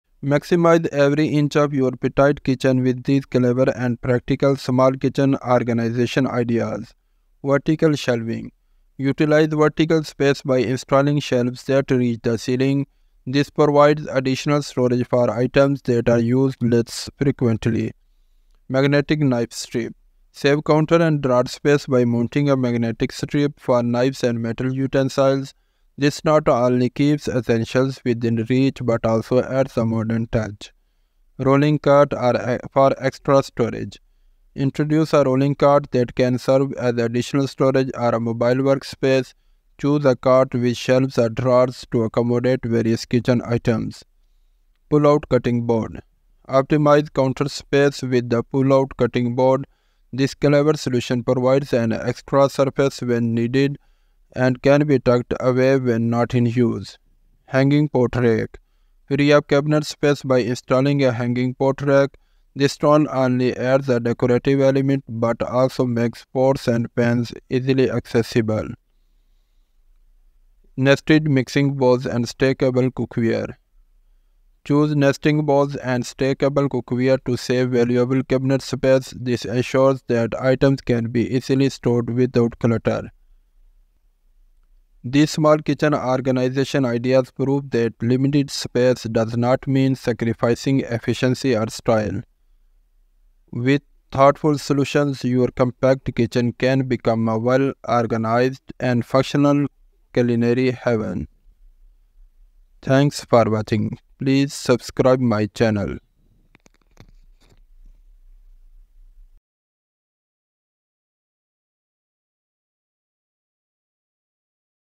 kitchen organization ideas | kitchen cabinet and storage organization ideas
Revamp your kitchen and reclaim your culinary space with our latest YouTube video, "Kitchen Organization Ideas"!  Explore ingenious solutions to declutter and optimize every nook and cranny of your kitchen. From vertical shelving and magnetic spice jars to clever pull-out pantry shelves, we've got the tips and tricks to make your kitchen a model of efficiency and style.
Join us on this visual journey to unlock the secrets of a well-organized and aesthetically pleasing culinary haven.